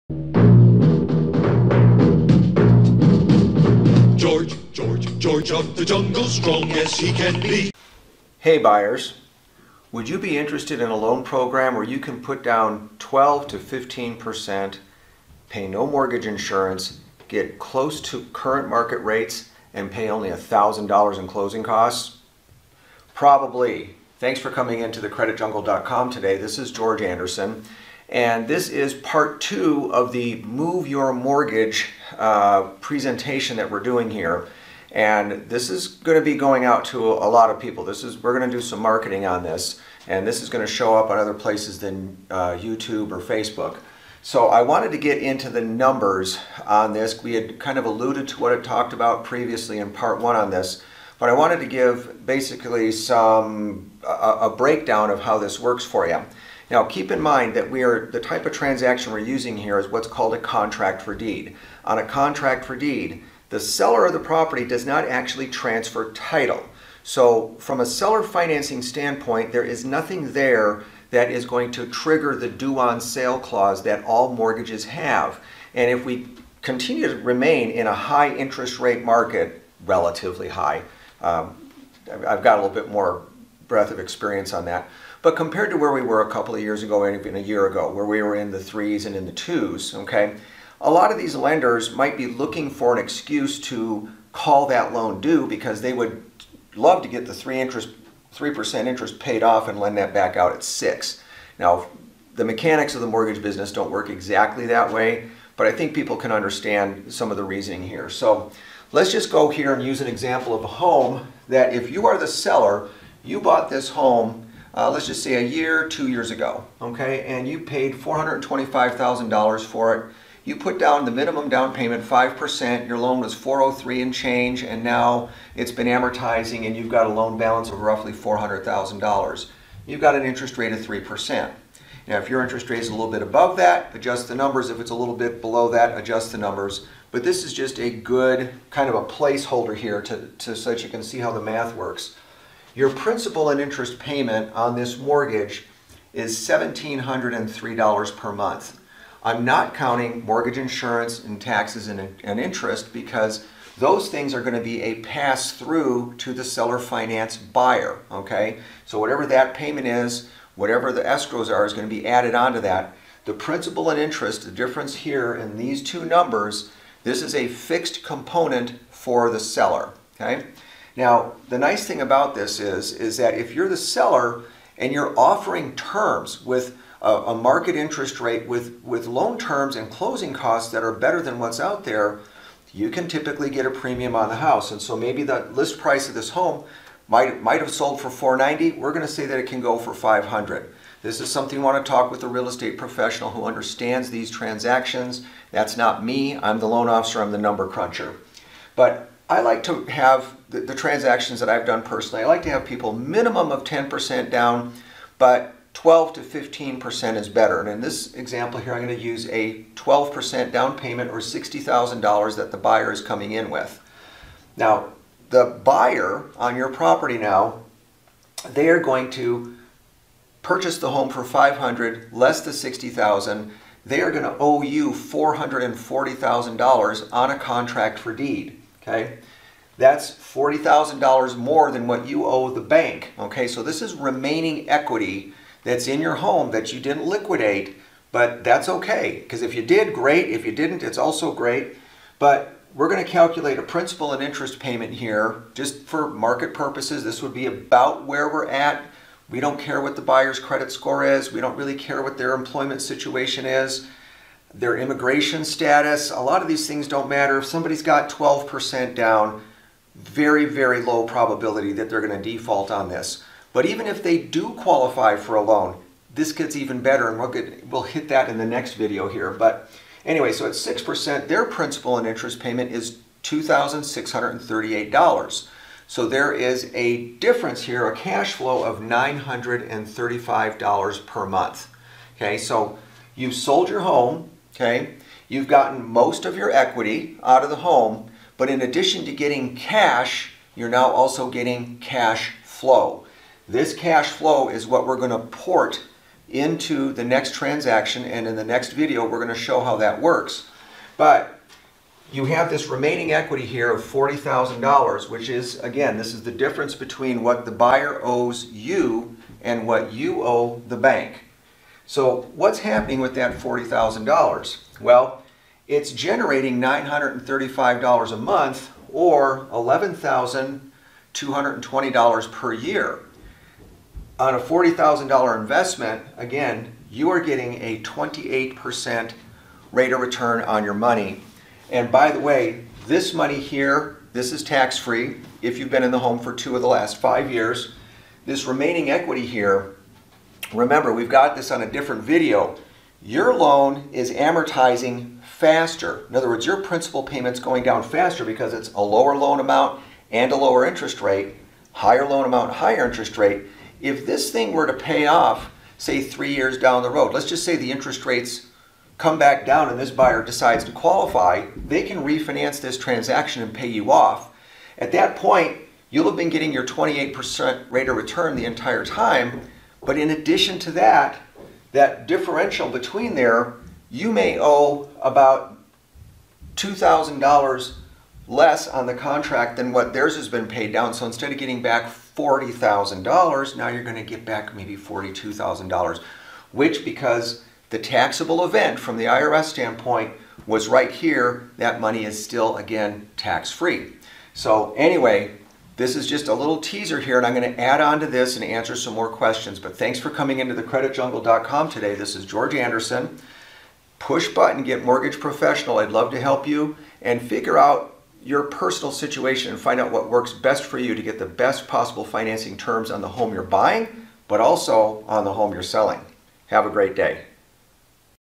Move Your Mortgage Part 2
Can you really move a mortgage in the same way it is done in some European countries?  Not exactly, but you can take the benefits of a low-interest rate on one home and take the savings to a new one.  This video uses a seller financing instrument known as a Contract for Deed.  It is called by different terms in different parts of the country, but "Contract for Deed" is the most recognizable term when referencing the guidelines for FHA, VA, Fannie Mae, and Freddie Mac.  This contract needs to be drafted by an attorney, followed by the recording of a Notice of Interest or Notice of Contract against the property.  The math for generating the cash flow is explained in this video.  The factors used to determine your cash flow and rate of return are based on 2 numbers.  First - the spread on the interest rate between your underlying mortgage and the buyer's contract for deed.  Second, the amount of equity you have remaining in your home.